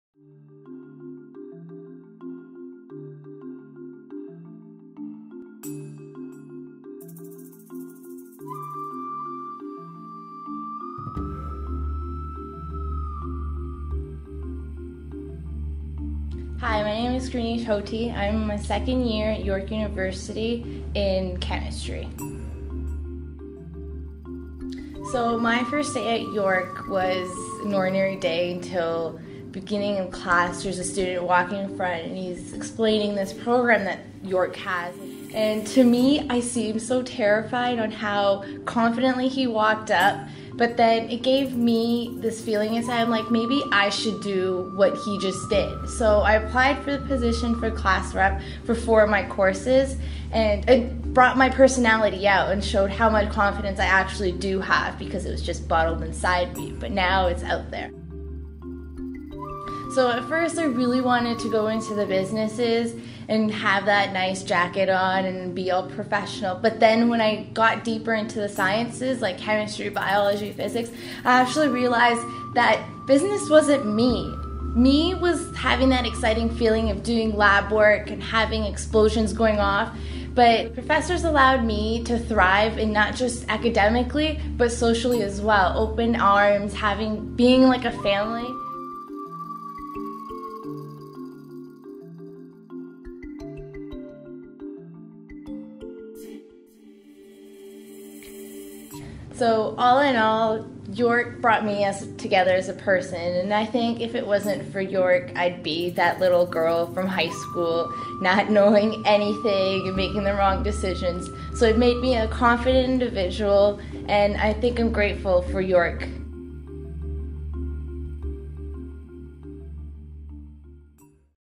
YorkU Student Profiles - Gurnish
Undergraduate Chemistry Student
Peer Mentor at Norman Bethune College